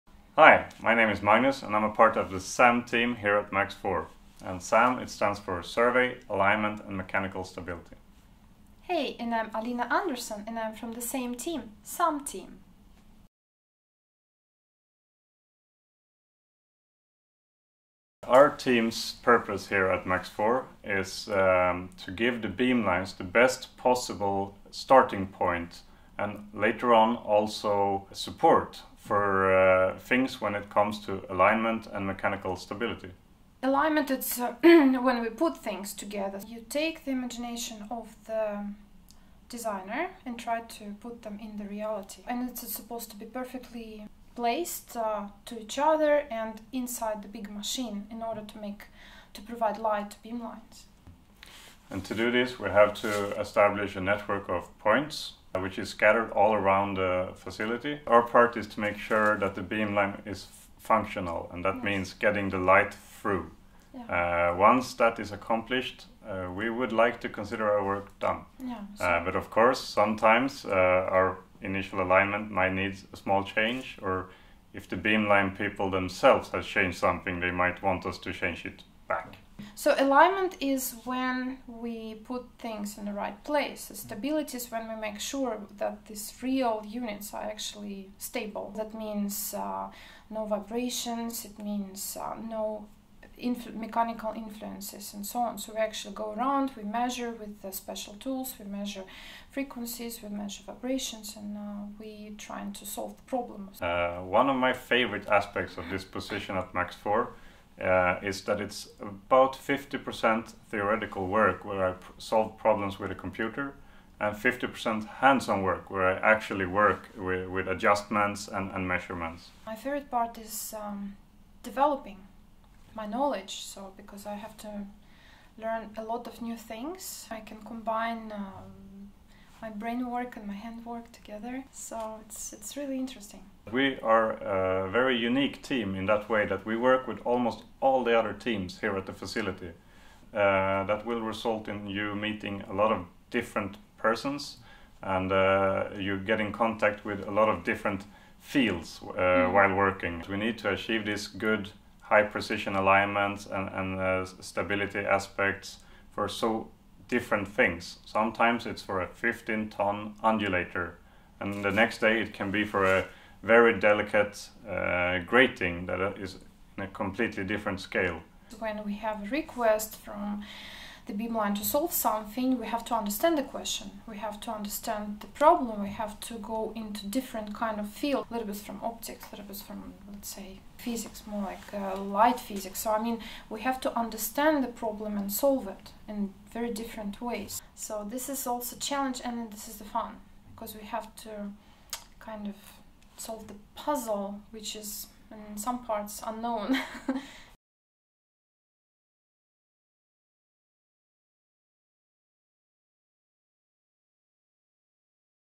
Magnus Malmgren and Alina Andersson - SAM team at MAX IV Laboratory
Meet Alina Andersson and Magnus Malmgren, engineers in the Survey, Alignment and Mechanical Stability team (SAM) at MAX IV. Alina and Magnus's mission at MAX IV is to make sure that the machines, instruments, and structures installed at the beamlines are perfectly aligned with the photon source, and that no vibrations compromise the analysis.
In their daily work, Alina and Magnus pass from using huge (and quite heavy!) wrenches, to handling wires 50 microns in diameter, as well as managing a complex map crucial for stability measurements and maintenance.